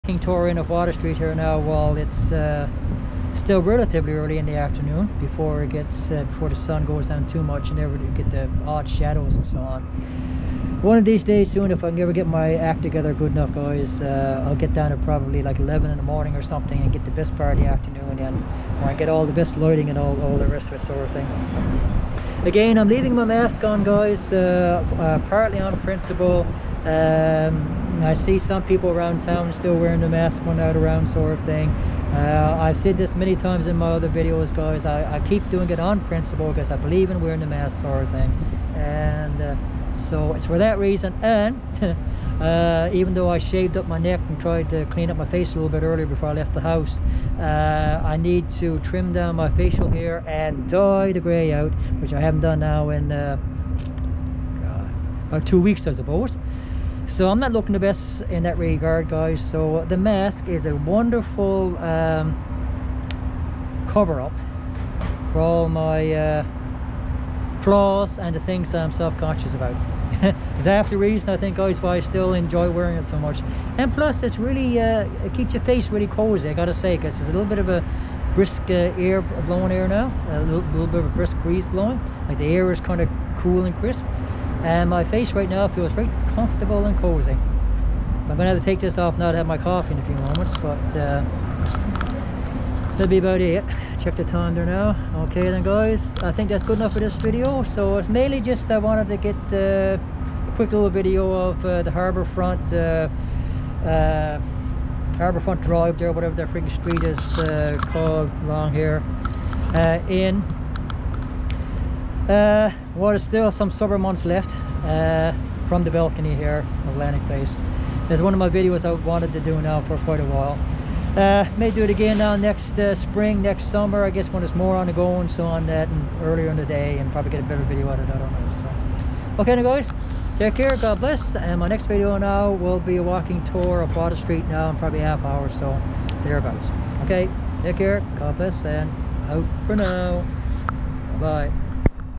Harbour Drive from Atlantic Place Balcony. Part 2 & End. Tuesday, September 21st, 2021.
Hey everyone. This is one of the videos I wanted to get in all Summer. Had hoped to get downtown earlier and get the harbour front in better light, but I never can seem to get things on the go early enough so this was the best I could do for now. I may try again, or wait til next Summer now. Please enjoy regardless.

Much Love. My Warmest Wishes. Be Well and Safe. Out For Now.